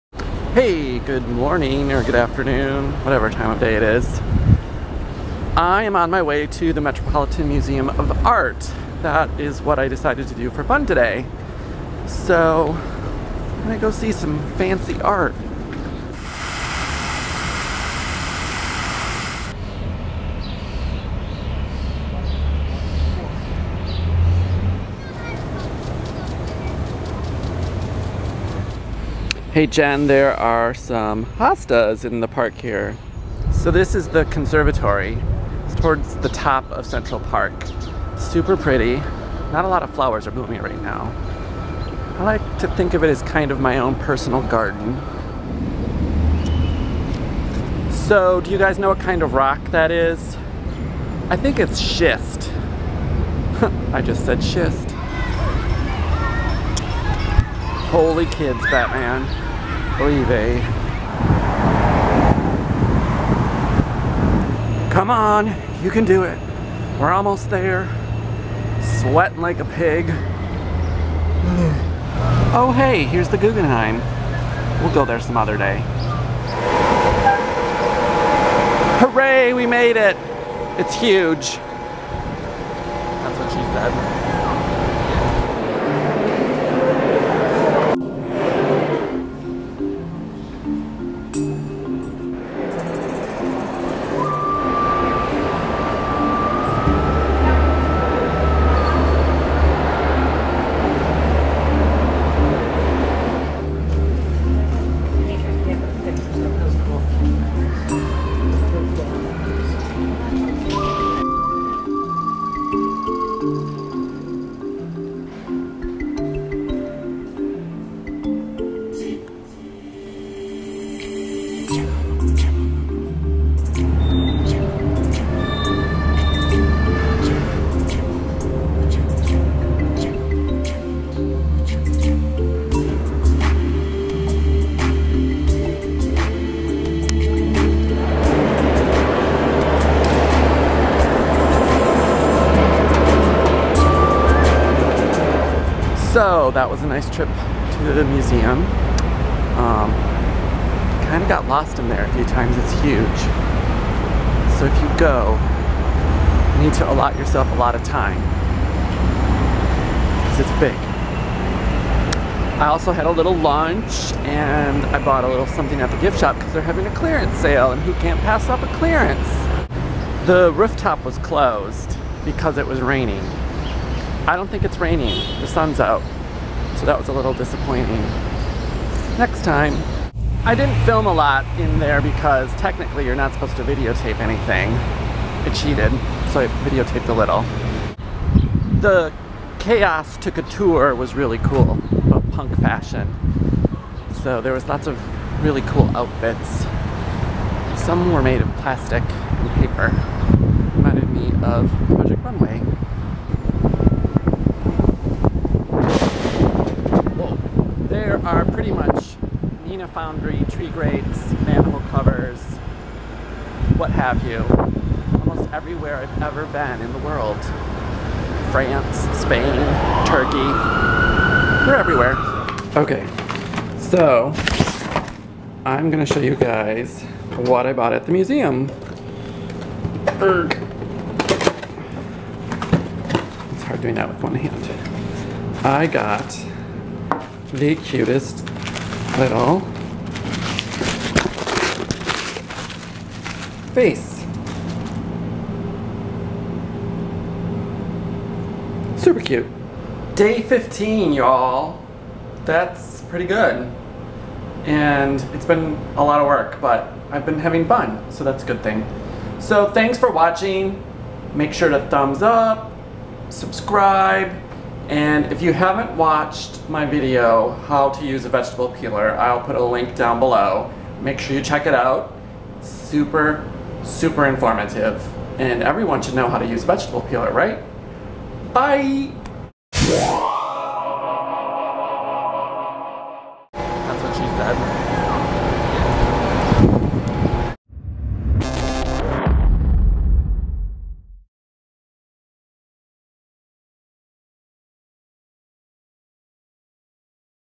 A Field Trip to The Metropolitan Museum of Art (07.10.13 | Day 15)
Join me on a field trip to the Metropolitan Museum of Art. On the way I strolled thru the Conservatory in Central Park, and see what I bought in the gift shop!